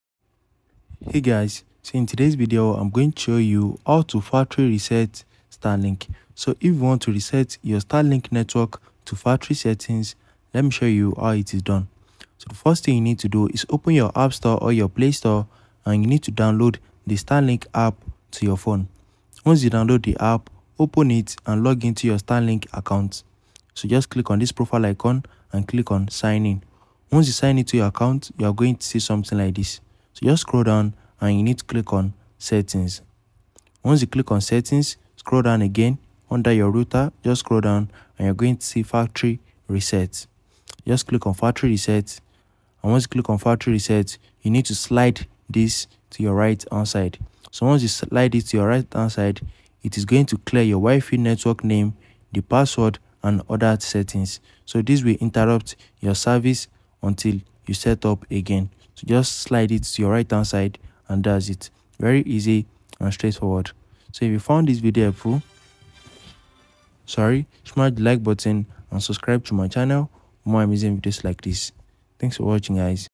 How to Factory Reset Starlink
Discover how to factory reset your Starlink hardware with our comprehensive tutorial! Whether you're troubleshooting issues or preparing to return your equipment, we'll guide you through the process step by step. Watch now to learn how to factory reset your Starlink device and restore it to its original settings!